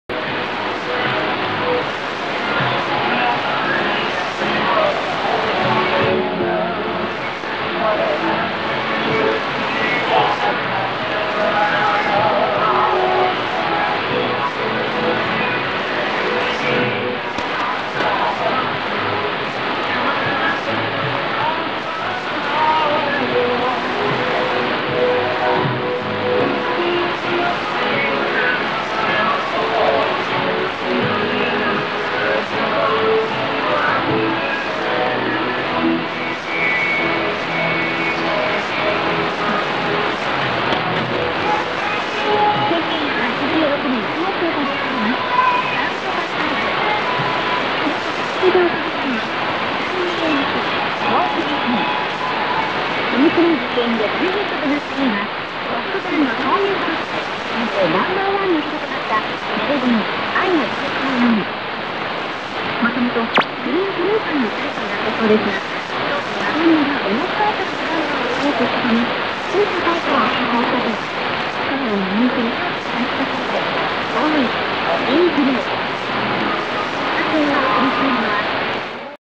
NHK World Radio Japan,2017 November 28, 07:25 UTC , 11825 kHz
2017 November 28, 07:25 UTC , 11825 kHz
NHK World Radio Japan
small signal, S3 R 2-3 severe Fading -2 and severe noise.
Via the 300KW Transmitter from Ibaragi-Koga / Japan
in japanese Language
Rec. in Germany JN49AC Loop R1000 QRB 9400km